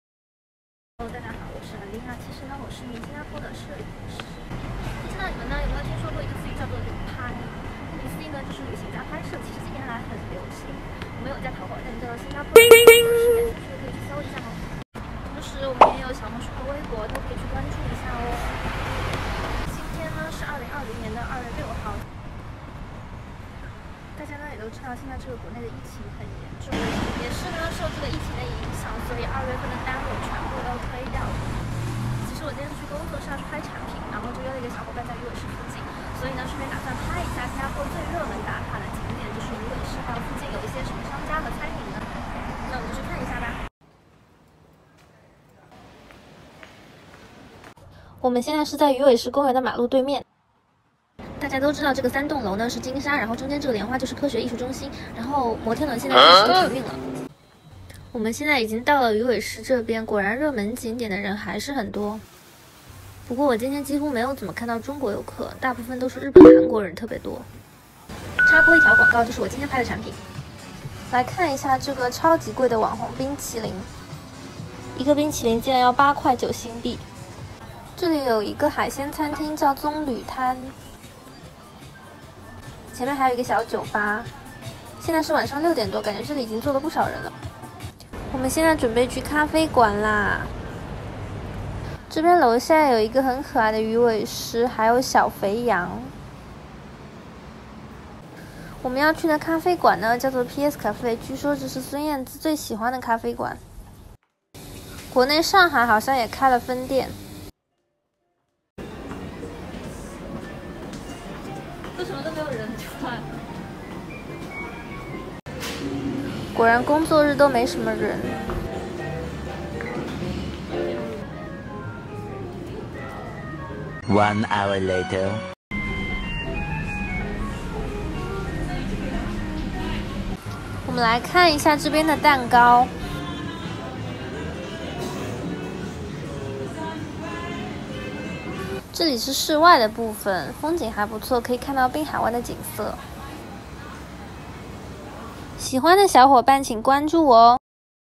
新加坡必打卡景点鱼尾狮💕孙燕姿最爱的咖啡馆
来新加坡一定要打卡的景点是哪里？孙燕姿最爱的咖啡馆又是哪一家呢